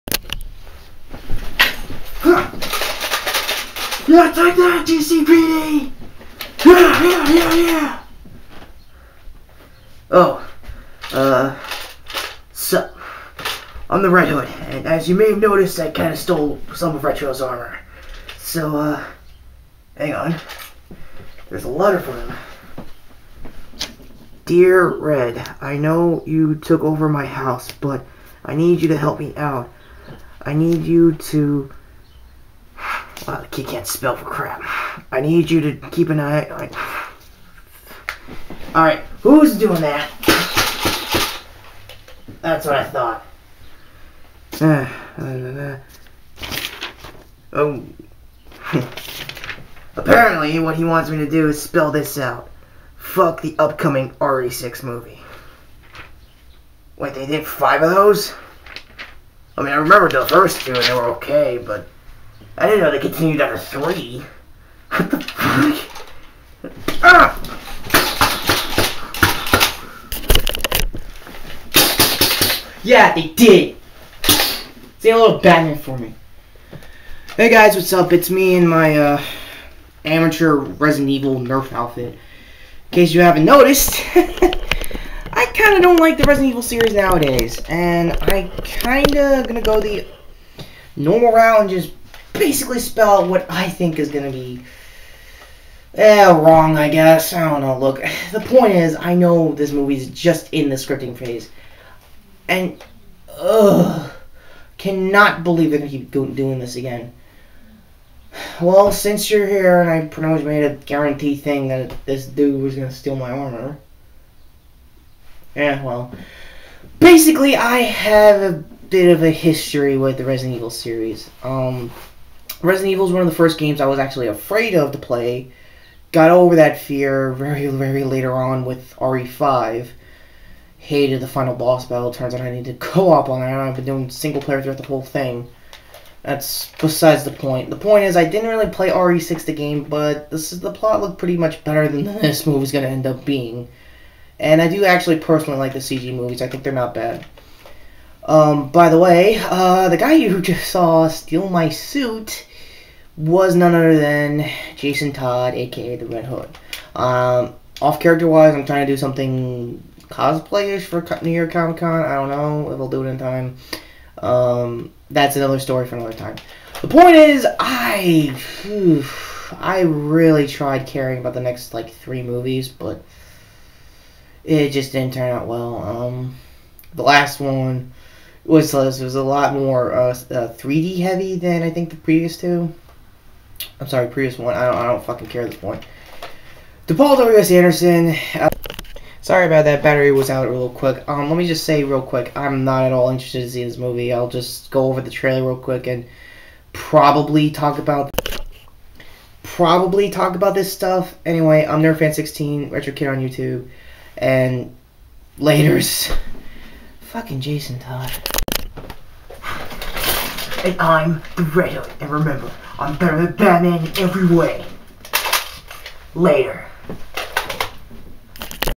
My RE 6 and Movies Rant Fet: RED HOOD
I created this video with the YouTube Video Editor and yes they are making another resident evil movie its only at the scripting phase but i thought it was a good time to talk about the movies at all cause i strait up dont care about them at all any more.